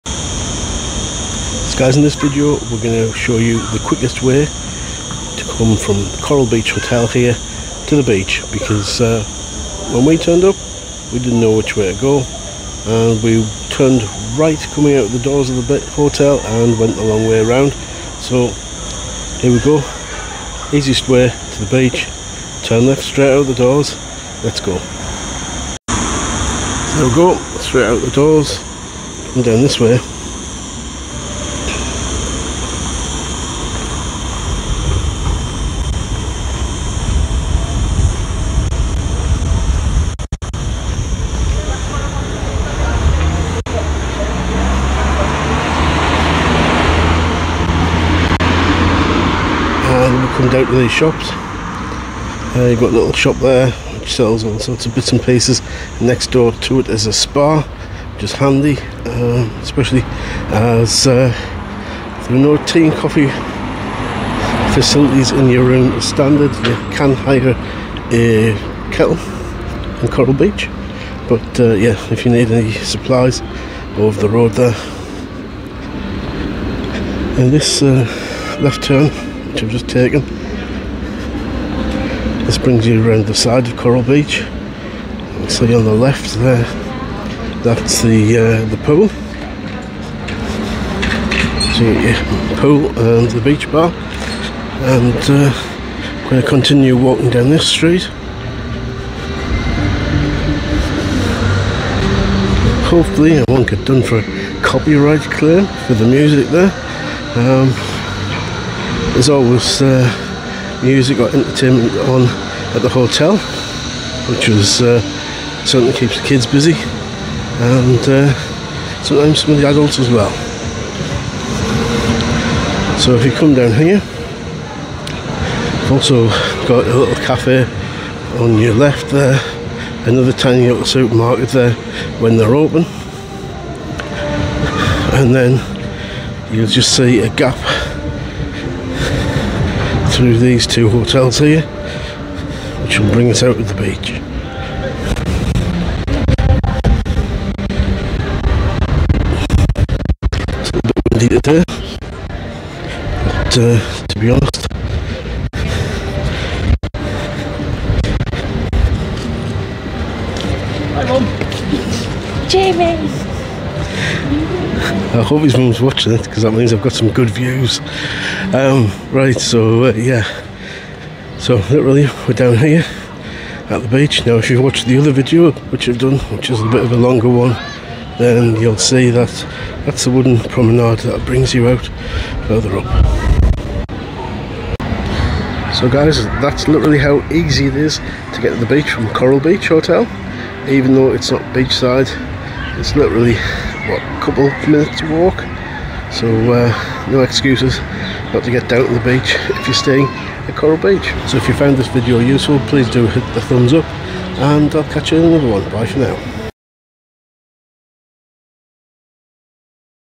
Getting from Coral Beach Hotel to Es Canar Beach
A simple video showing the 2 to 3 minute walk from the Coral Beach Hotel to the beach in the holiday resort of Es Canar in Ibiza, Spain.